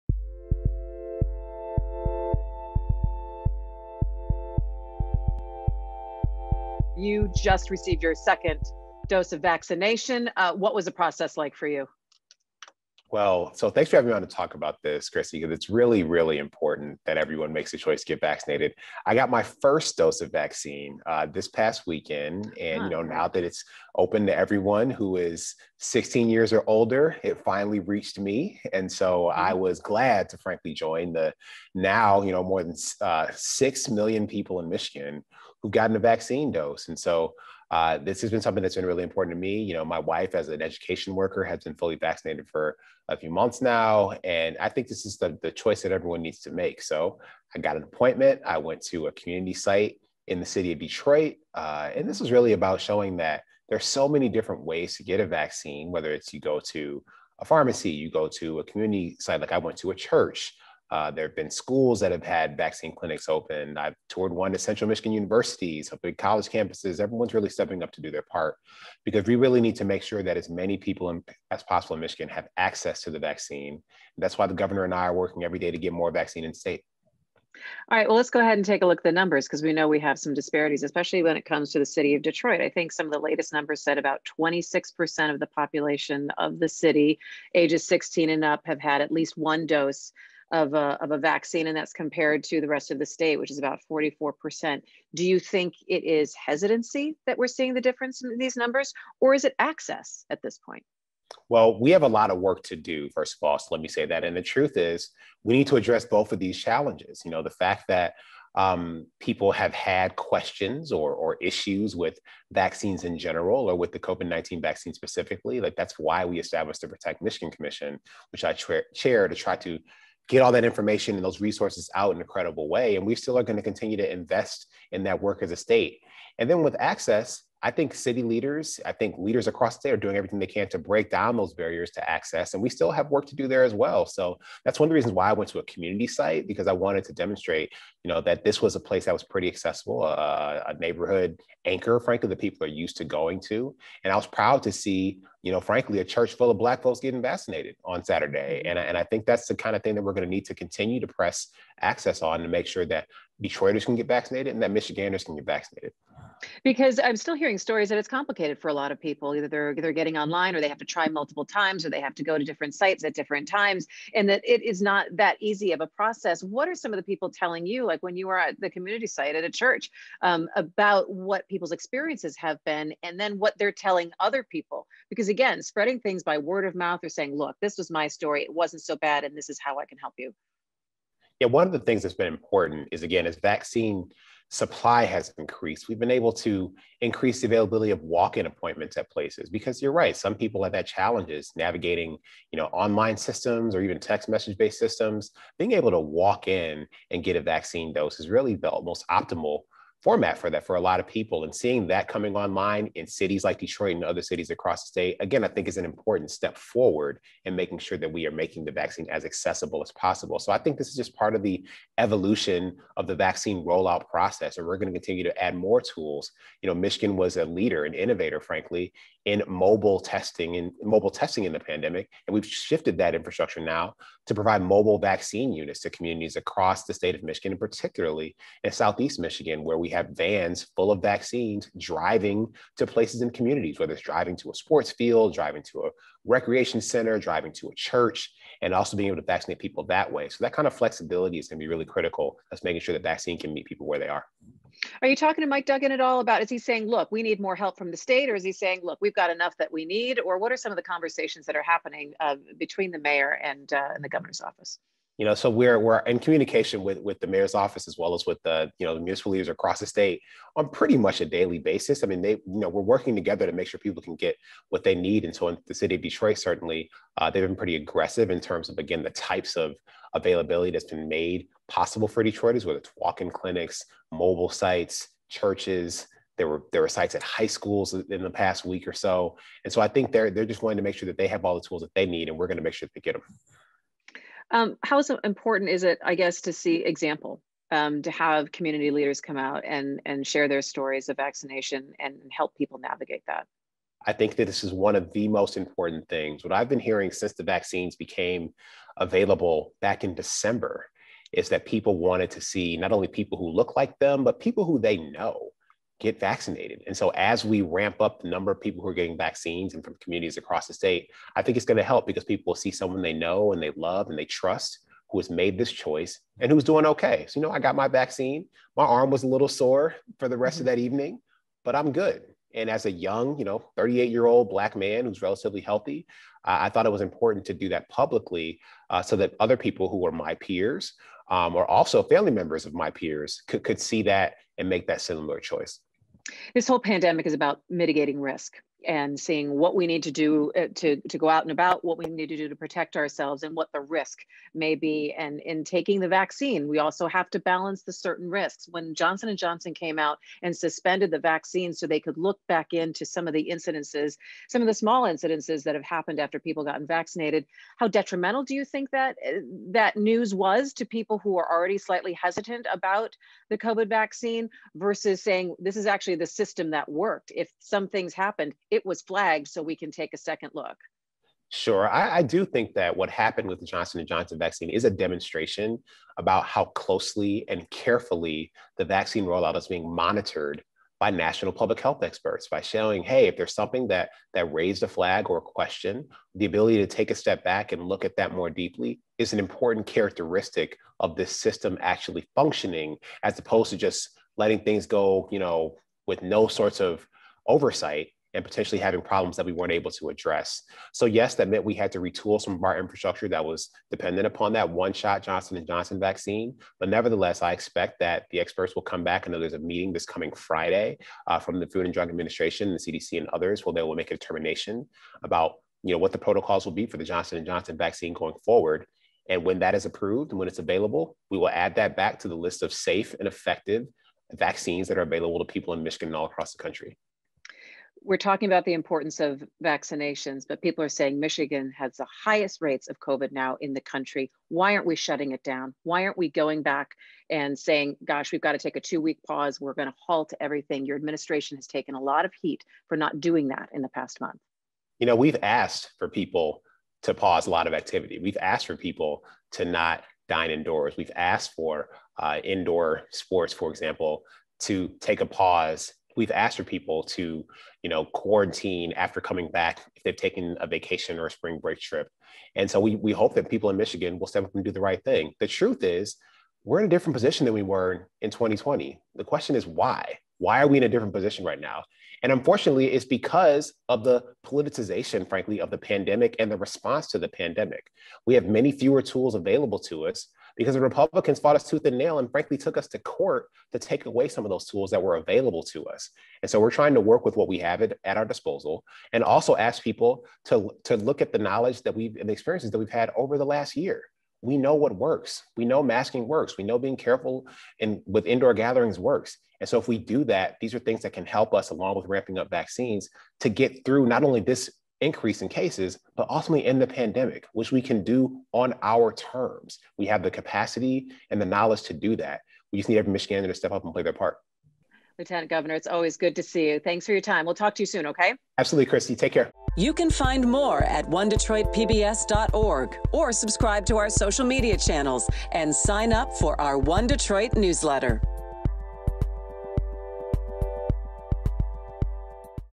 Lt. Gov. Garlin Gilchrist on Vaccine Access and Vaccine Hesitation
Christy McDonald talks with Lt. Governor Garlin Gilchrist about the challenges of vaccine access, addressing vaccination hesitation in the city of Detroit, and answering the question of why the Governor’s office hasn’t made the decision to enact another shutdown. Check out the full interview here.